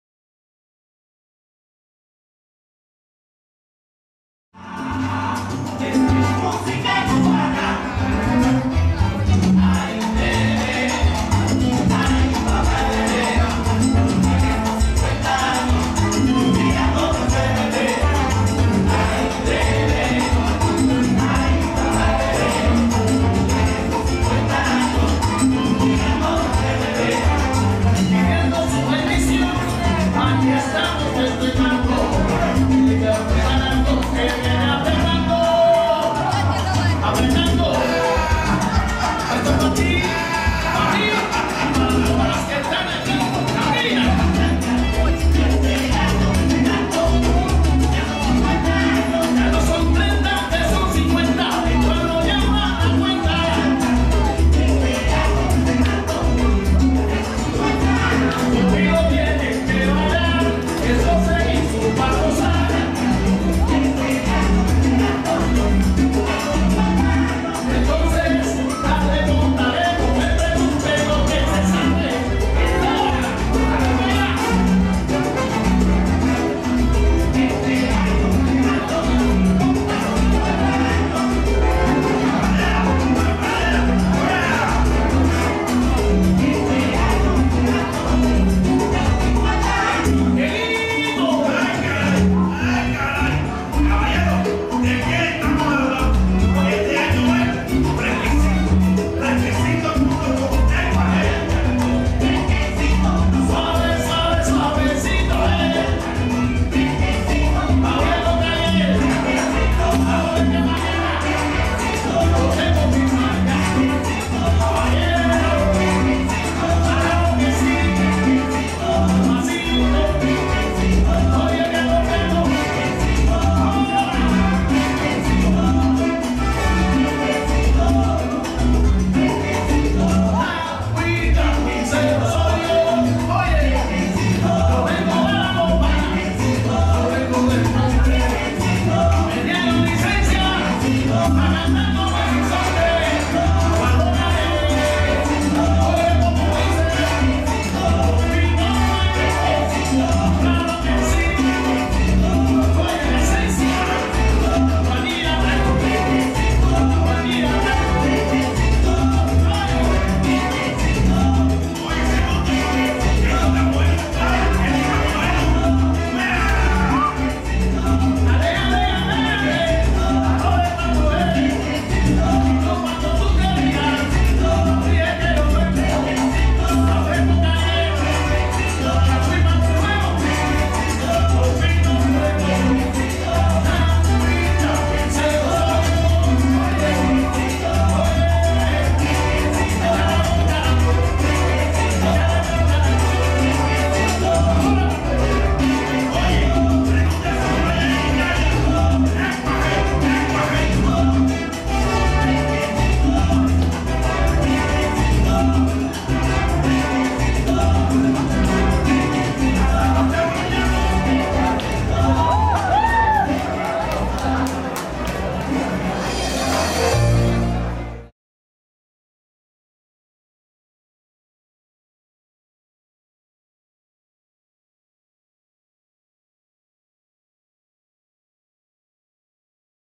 Yoannis Tamayo and Svetlana Levchenko Salsa Dancing at Respublika Days 9, Sunday 05.05.2019 (SC)
Yoannis Tamayo and Svetlana Levchenko Salsa Dancing at Respublika Days 9 (Rostov-on-Don/Russia), Sunday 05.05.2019 (SC)